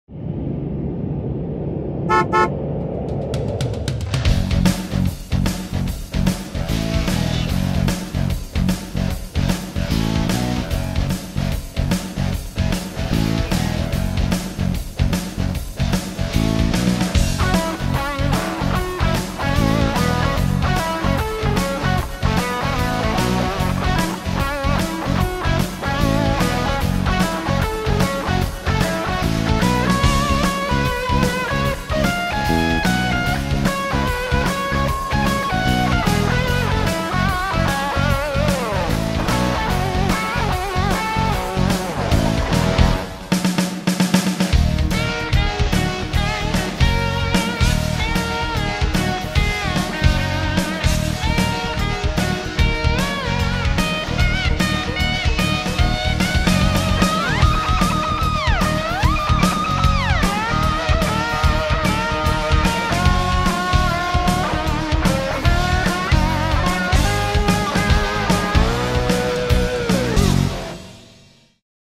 Rock and Roll at de-ice Oslo Airport Norway
Heavy snow made this day quite interesting at de-ice plate alpha south. Recorded Sunday 27 December 2015